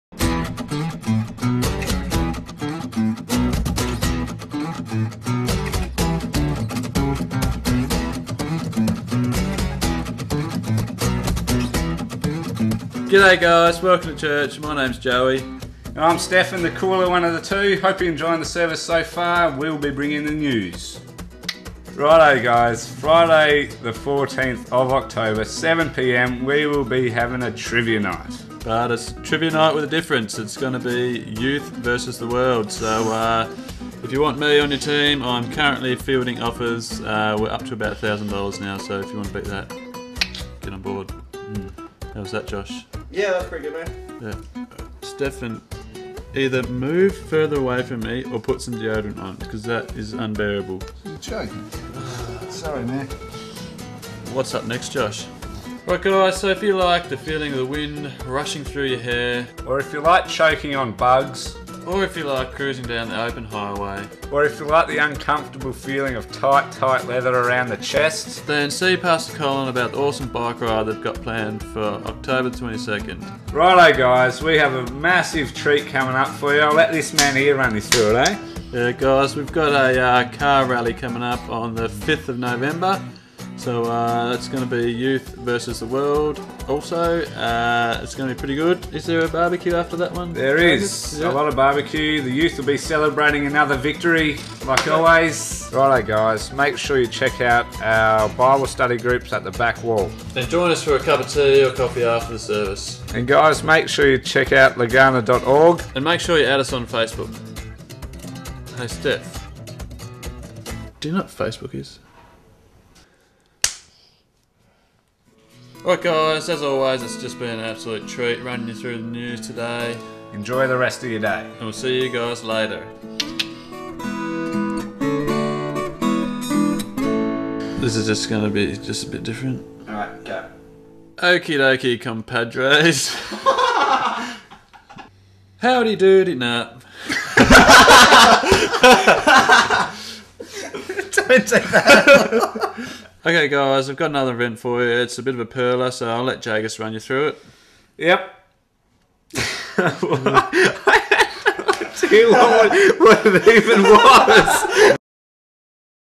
Upcoming News & Events - September
Joe Hammond and Stefan Jago present the Church News for Legana Christian Church,